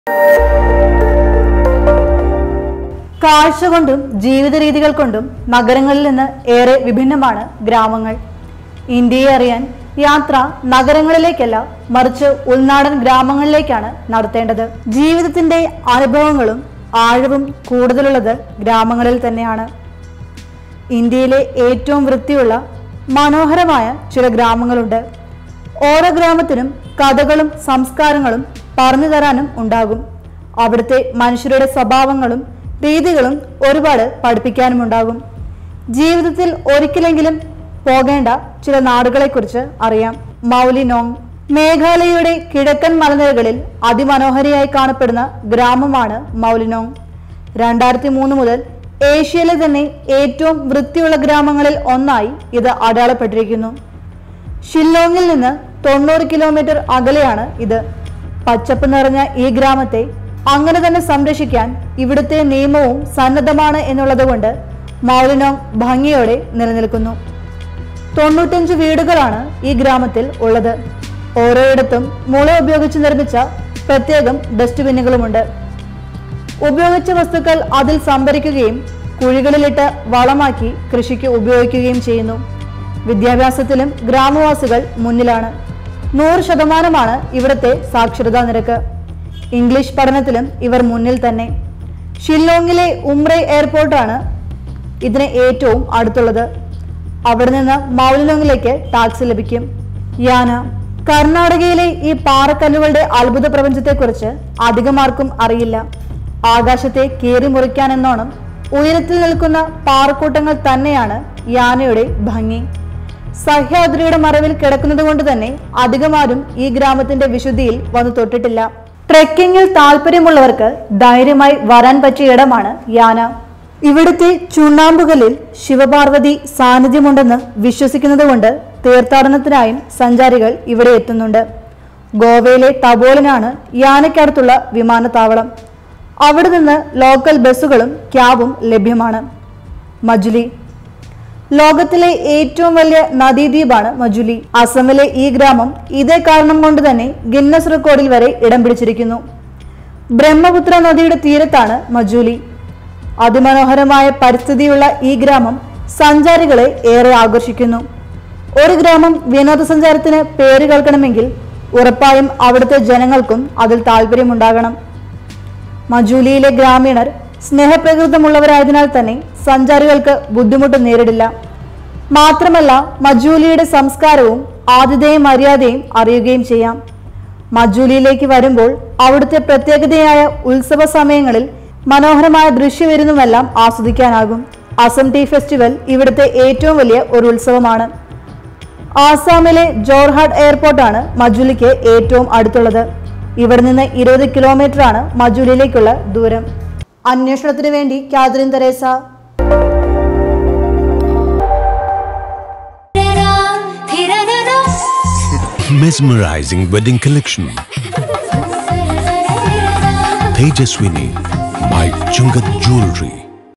ഇന്ത്യൻ ഉൾഗ്രാമങ്ങളിലൂടെ
കാഴ്ചകൊണ്ടും ജീവിതരീതികൾ കൊണ്ടും നഗരങ്ങളിൽനിന്ന് ഏറെ വിഭിന്നമാണ്‌ ഗ്രാമങ്ങൾ. ജീവിതത്തിന്റെ അനുഭവങ്ങളും ആഴവും കൂടുതലുള്ളത് ഗ്രാമങ്ങളിൽത്തന്നെയാണ്. ഇന്ത്യയെ അറിയാൻ യാത്ര നഗരങ്ങളിലേക്കല്ല, ഉൾഗ്രാമങ്ങളിലേക്കു തന്നെയാവണം. ഇന്ത്യയിലെ ഏറ്റവും വൃത്തിയുള്ള, മനോഹരമായ ചില ഗ്രാമങ്ങളുണ്ട്. ഓരോ ഗ്രാമത്തിനും കഥകളും സംസ്കാരങ്ങളും പറഞ്ഞു തരാനുണ്ടാകും, അവിടുത്തെ മനുഷ്യരുടെ സ്വഭാവങ്ങളും രീതികളും ഒരുപാട് പഠിപ്പിക്കാനുമുണ്ടാകും. ജീവിതത്തിൽ ഒരിക്കലെങ്കിലും ഈ സ്ഥലങ്ങളിൽ പോകണം.

മൗലിനൊങ്

മേഘാലയയുടെ കിഴക്കൻ മലനിരകളിൽ അതിമനോഹരിയായി കാണപ്പെടുന്ന ഗ്രാമം. 2003 മുതൽ ഏഷ്യയിലെ തന്നെ ഏറ്റവും വൃത്തിയുള്ള ഗ്രാമങ്ങളിൽ ഒന്നായി ഇത് അടയാളപ്പെട്ടിരിക്കുന്നു. ഷില്ലോങ്ങിൽനിന്ന് 90 കിലോമീറ്റർ അകലെയാണിത്. പുഷ്പ, ലതാ സമൃദ്ധമായ, പച്ചപ്പു നിറഞ്ഞ ഈ ഗ്രാമത്തെ അങ്ങനെതന്നെ സംരക്ഷിക്കാൻ ഇവിടുത്തെ നിയമവും സന്നദ്ധമാണ് എന്നുള്ളതുകൊണ്ട് മൗലിനൊങ് ഭംഗിയോടെ നിലനിൽക്കുന്നത്.